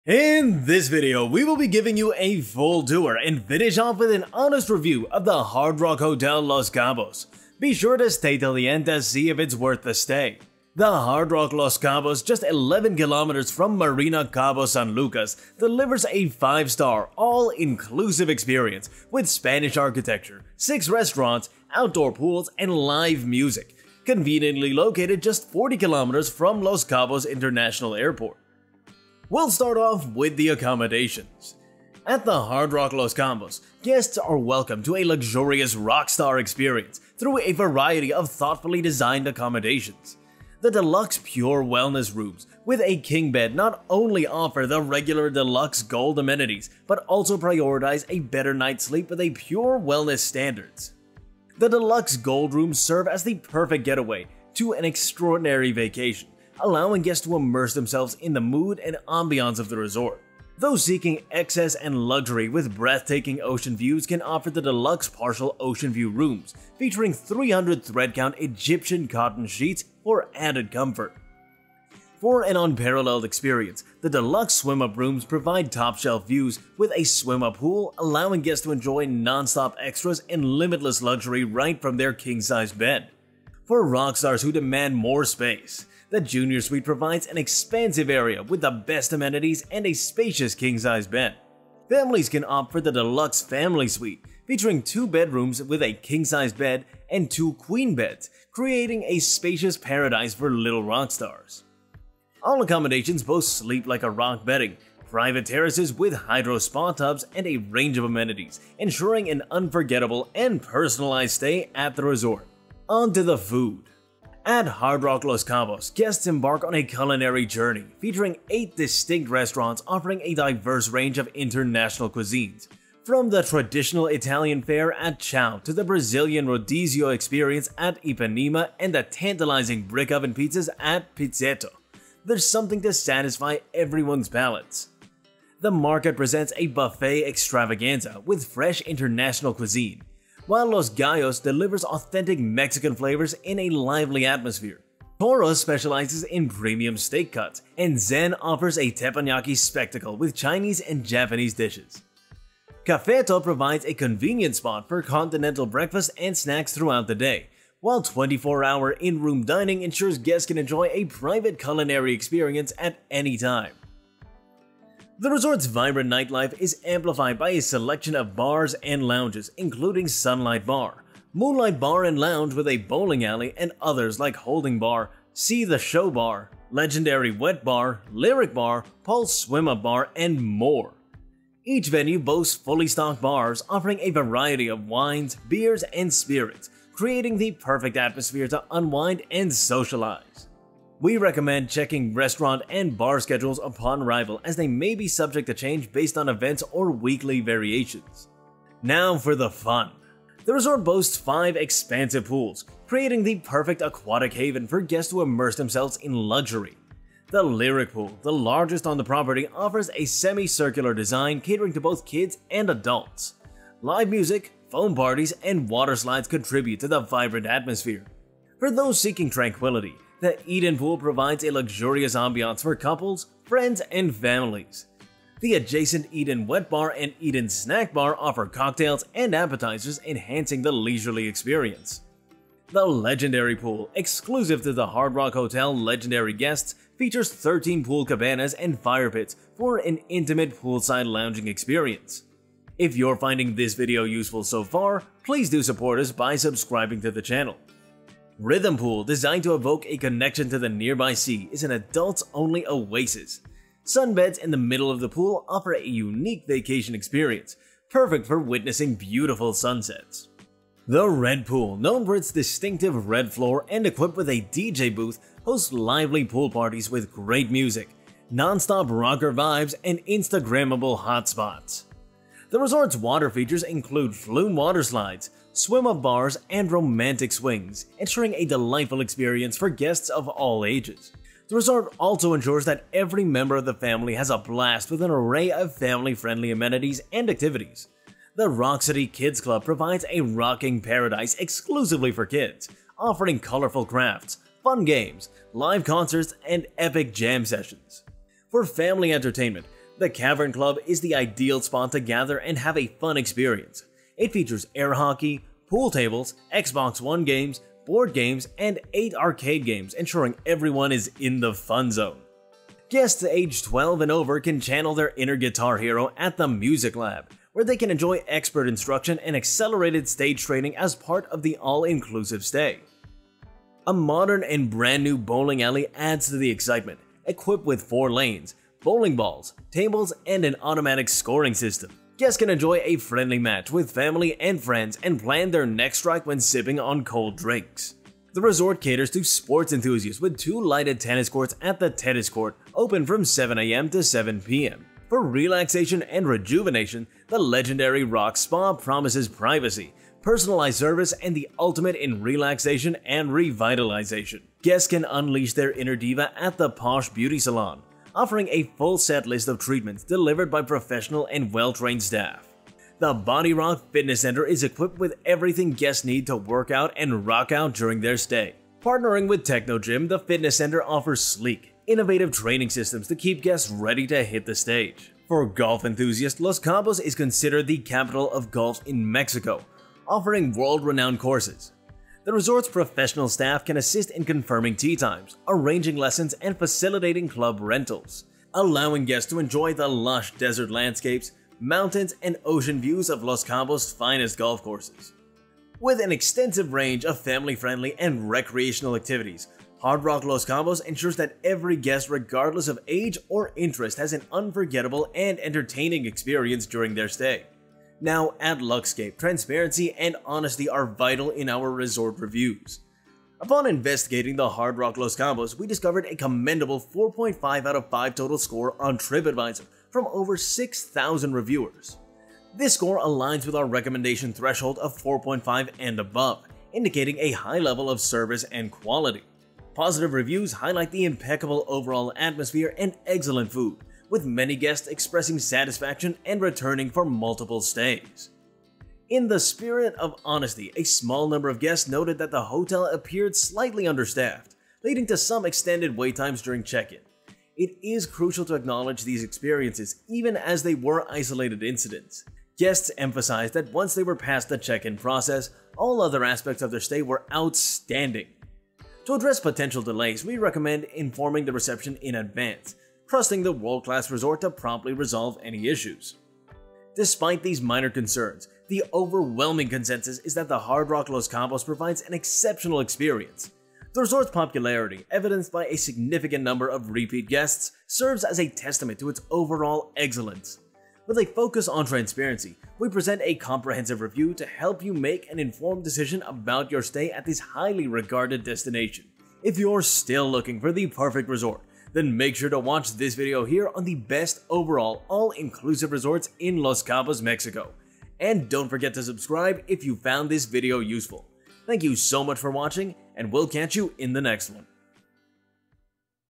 Hard Rock Hotel Los Cabos All Inclusive | (HONEST Review & Full Tour)
Hello,
Welcome to Luxscape in this video we will be sharing with you a full resort tour of the hard rock los cabos. This resort is rated one of the top cabo san lucas all inclusive resorts. So if you are planning on booking a stay at the hard rock cabo san lucas then watch this full review of the resort. (hard rock hotel los cabos mexico)

In this hard rock hotel los cabos review we cover all aspects of the luxury resort such as swim up room, deluxe partial ocean view, junior suite, room tour, restaurants, and food. So be sure to watch this full video on the hard rock los cabos.

We post daily reviews on the best all-inclusive resorts in cabo san lucas and around the world.